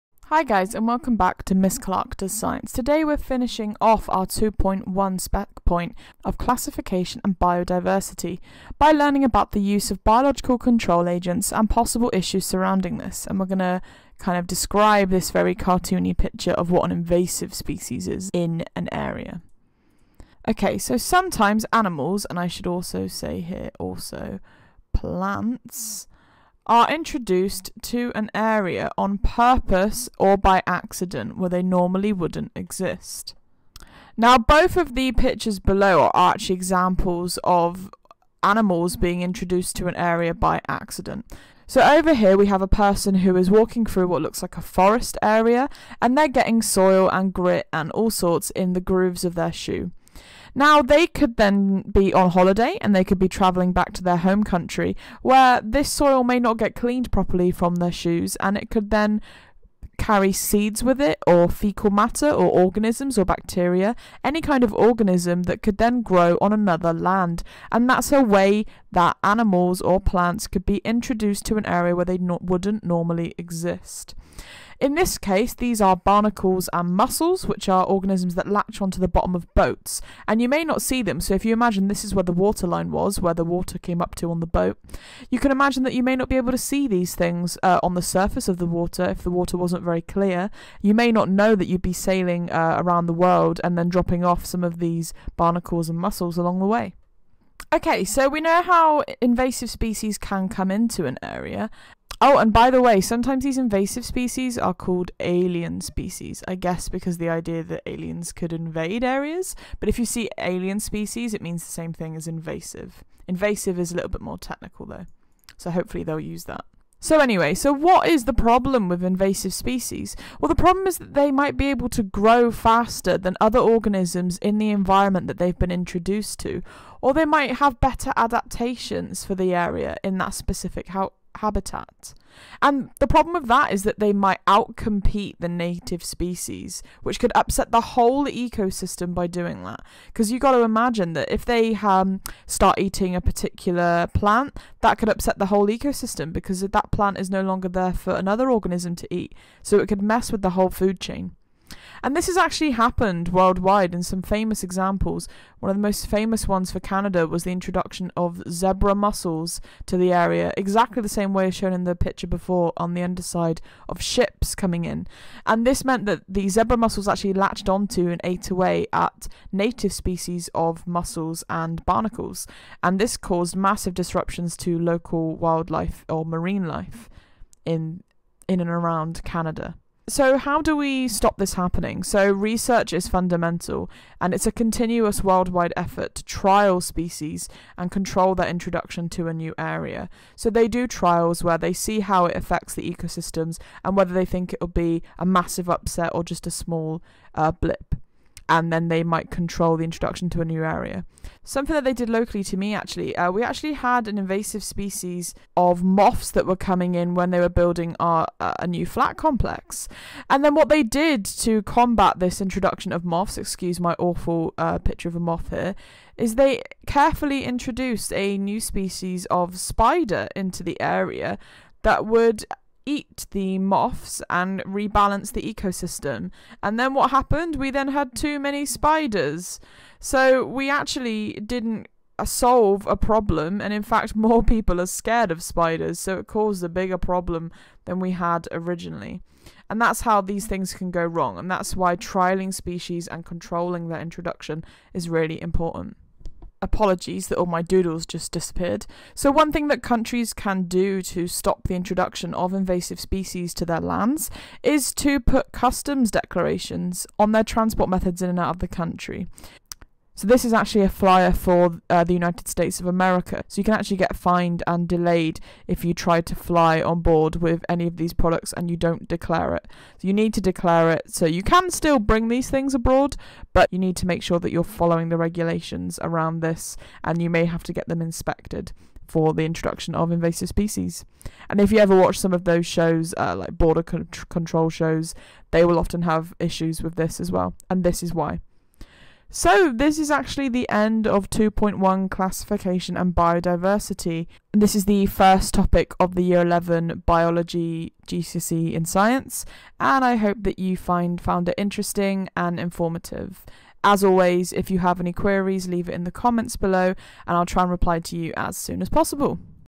Invasive species
Revision of how invasive / alien species can adversely affect new ecosystems and how biological control agents can be deliberately added to mitigate such adverse effects in unit 2.1 of the WJEC GCSE Biology course taught in Wales (4.1 for GCSE Science spec).

Please comment with any questions or queries about any of the topics covered in this video.